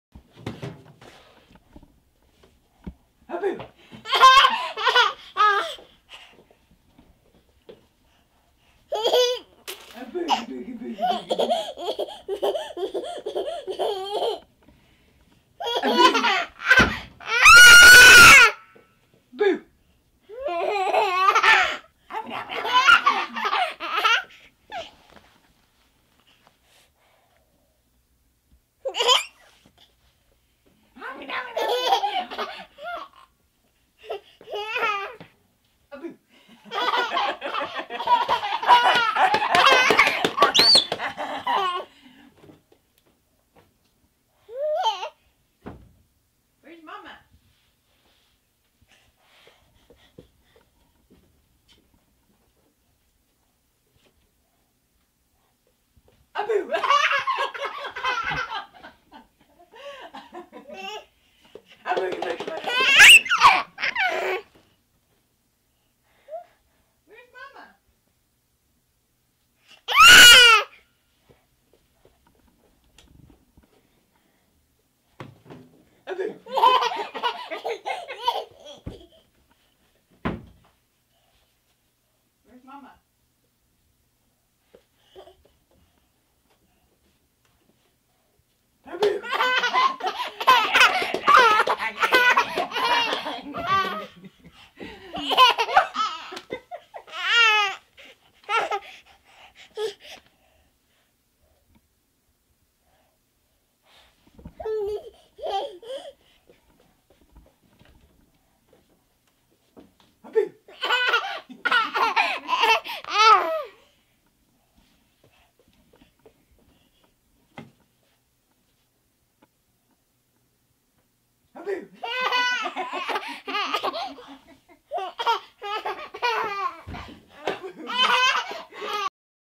The Life of Pixel - Where's Mommy?
Pixel plays hide and seek with Mommy.